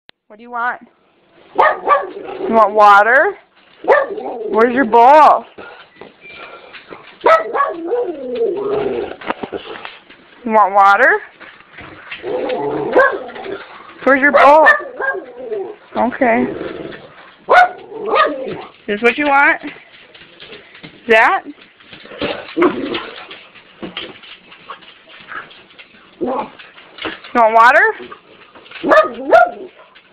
Schnoodles and Weimaraner, asking for a drink!
Funny video of dogs. Mini schnoodle, Pixi, barks telling me she wants water. Mini schoodle, Tink (Pixi's sister), patiently waits. Weimaraner, Ace, well. she thinks she is a little dog too! lol

Yes, I did in fact give them water. And please trust me, they were not dying of thirst. lol

Sorry about the poor quality and short time, was taken with my cell phone.